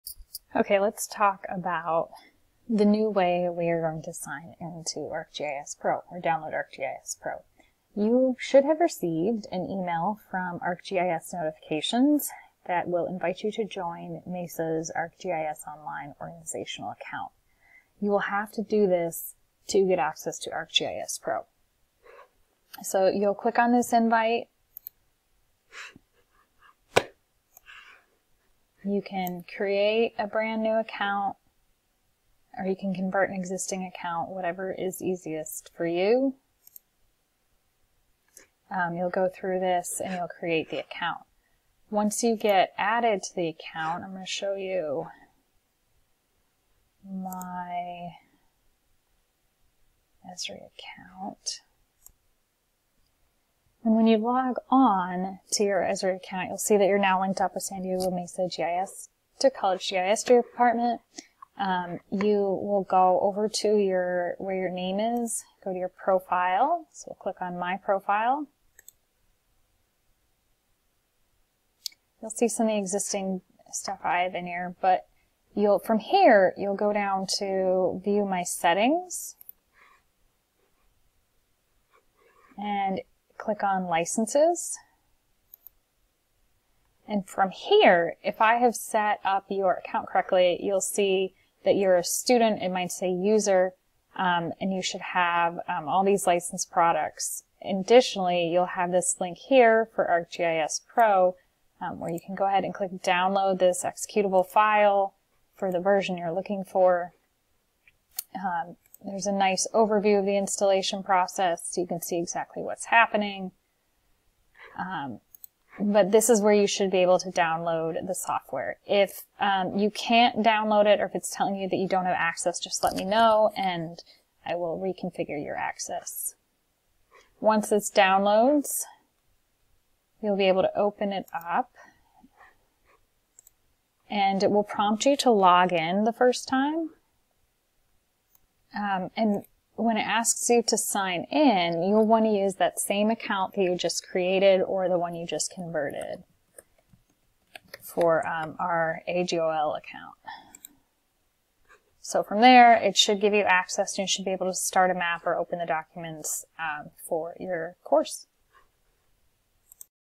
How to Install ArcGIS Pro Spring 2021
New way of downloading ArcGIS Pro from your ArcGIS Online Organizational Account